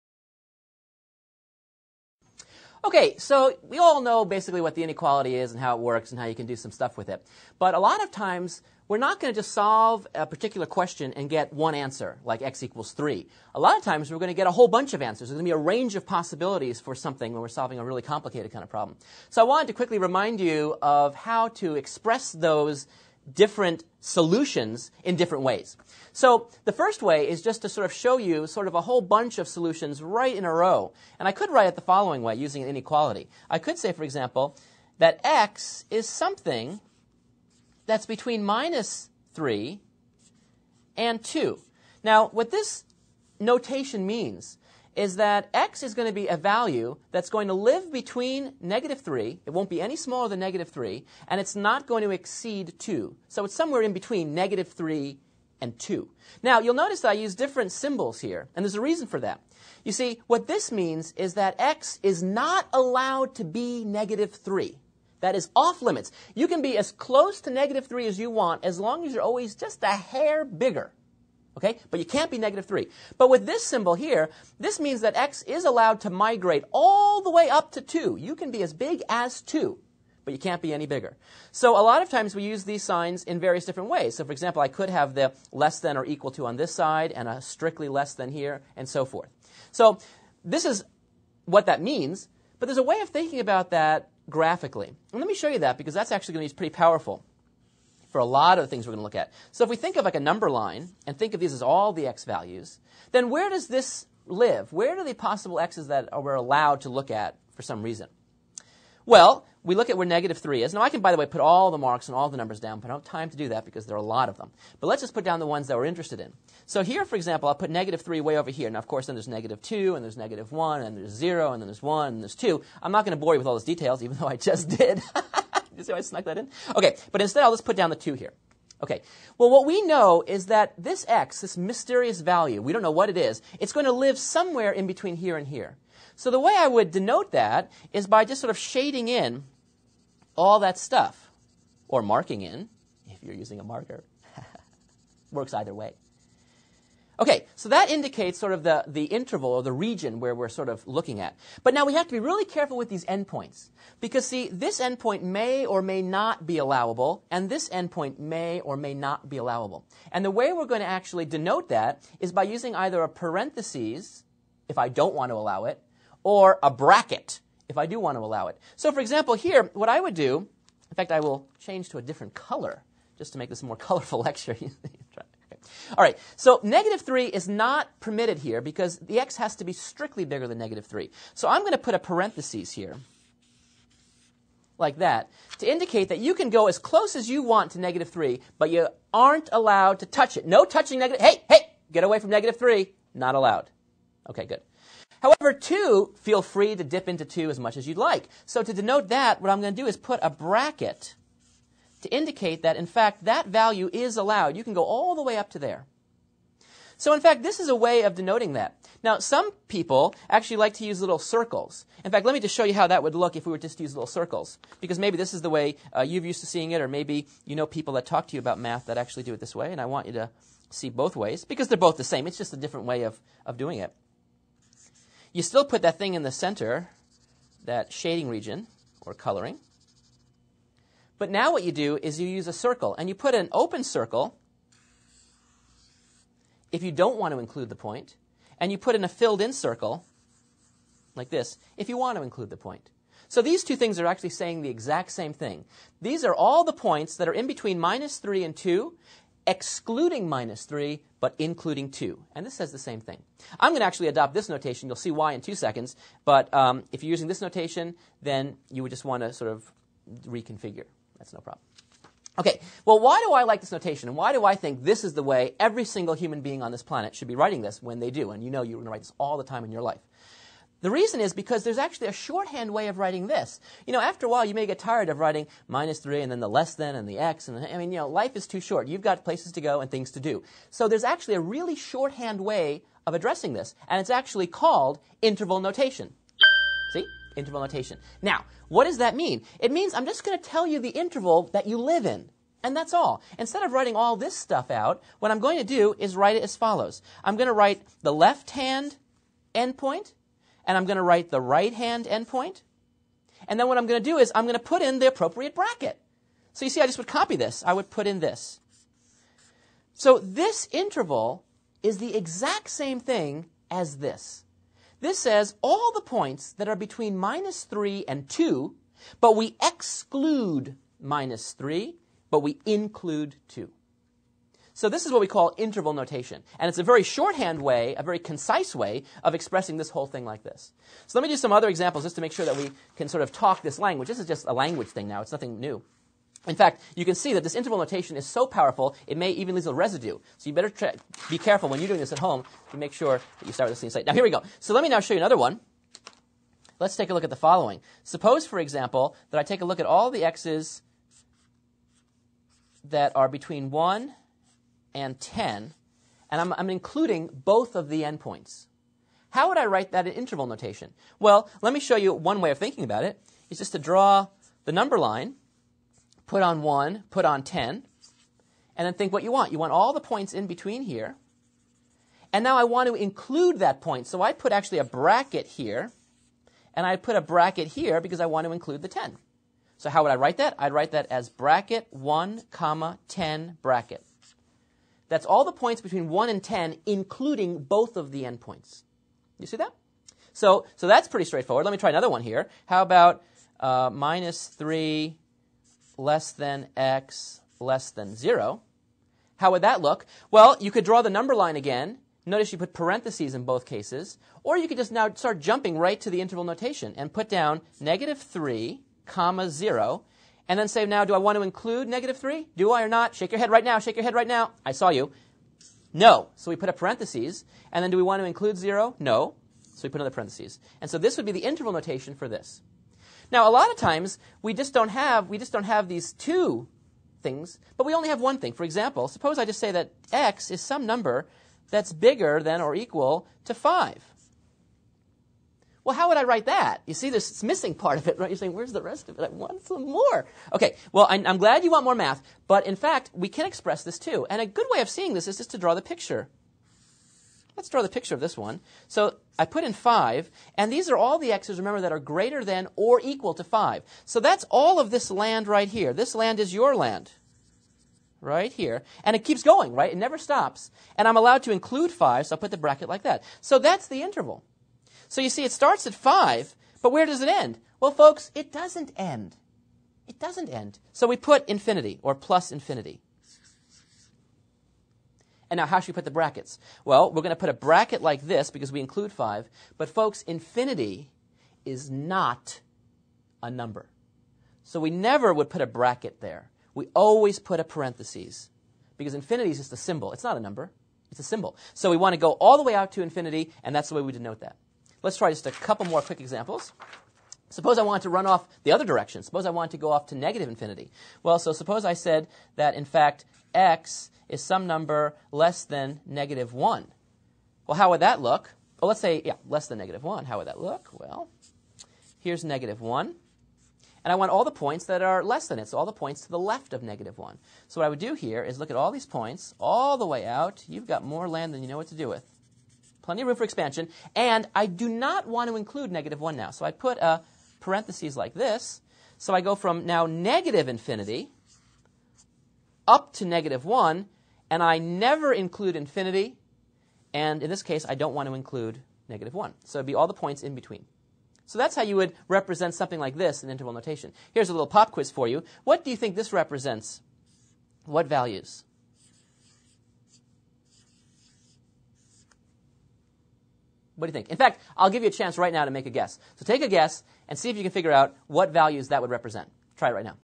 Inequalities and Interval Notation, Part 1 of 2, from Thinkwell's College Algebra
Award-winning Professor Edward Burger discusses how to solve inequalities and express their solutions in interval notation.